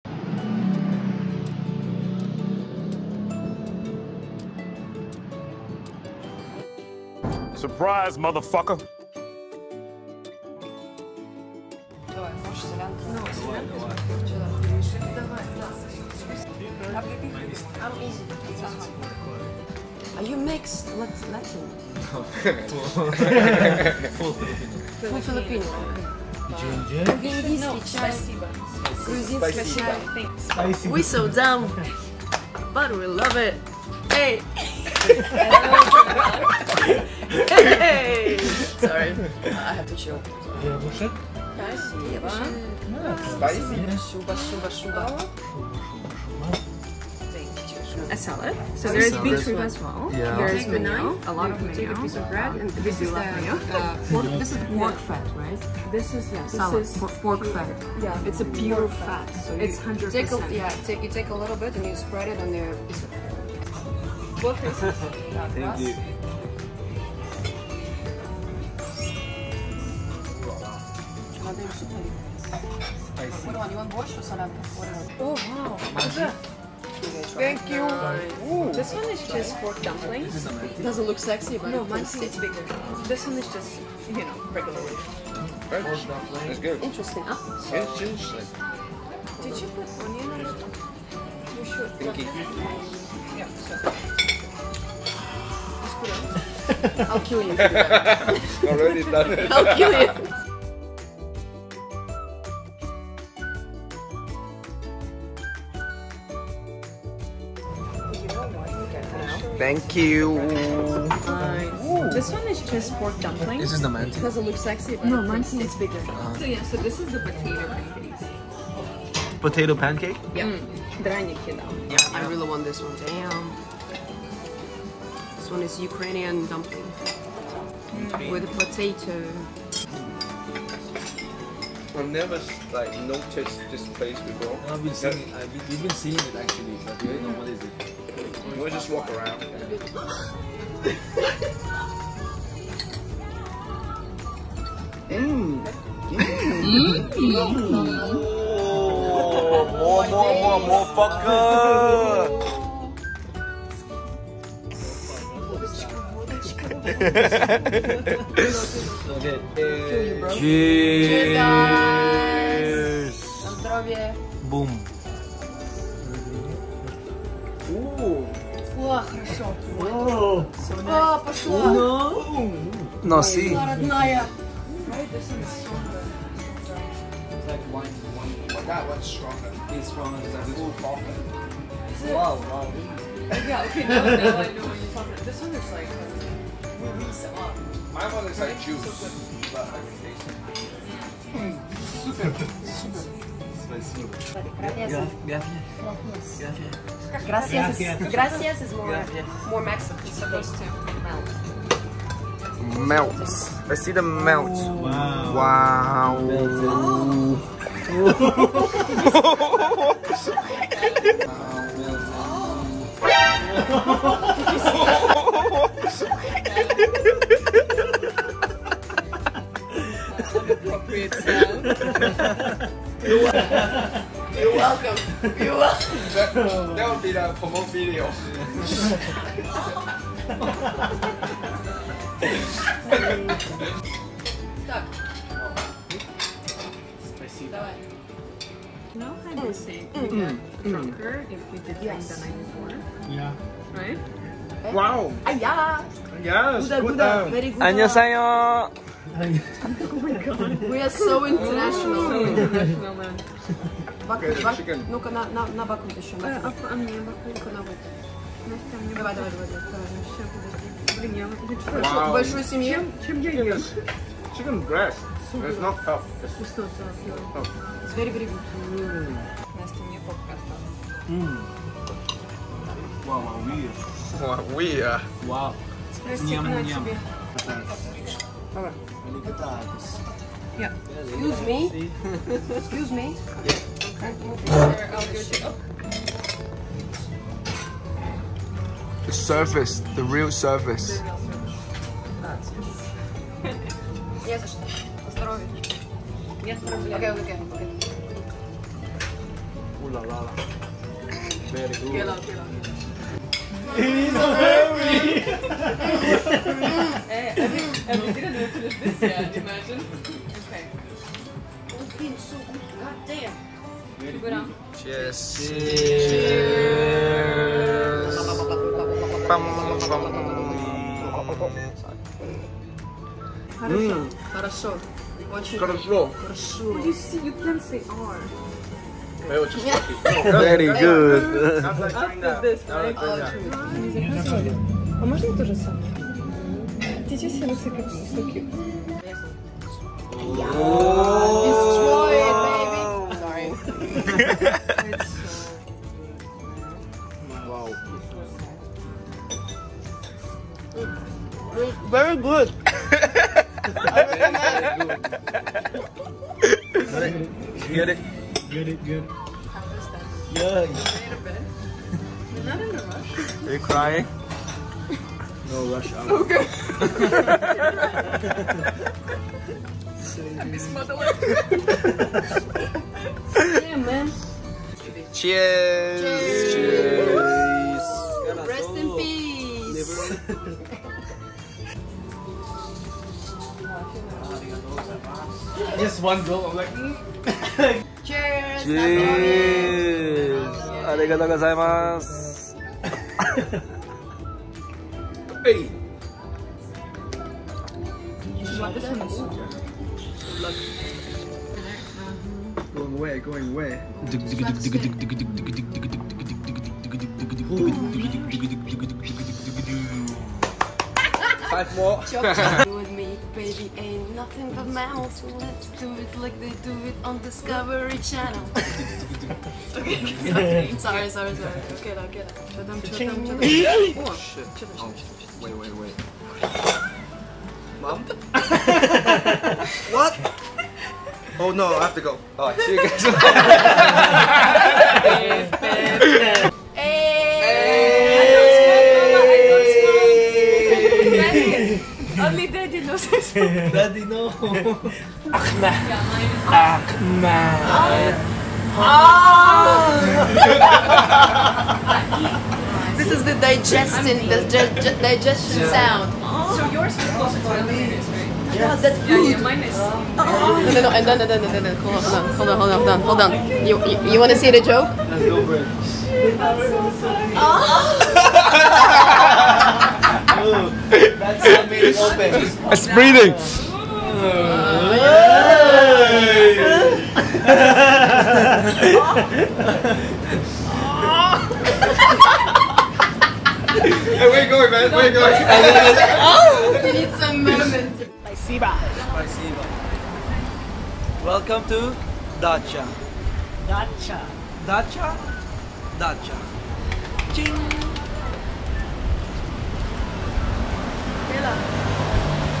First Ukrainian food experience in Hong Kong!! w/ Special guests
Restaurant details
Dacha Restaurant & Bar

Yana - yanayes
Nastya - nastymeteva
Chows - charlesjoseph25
Jaycup - jakesalooooonga
Me - mancburger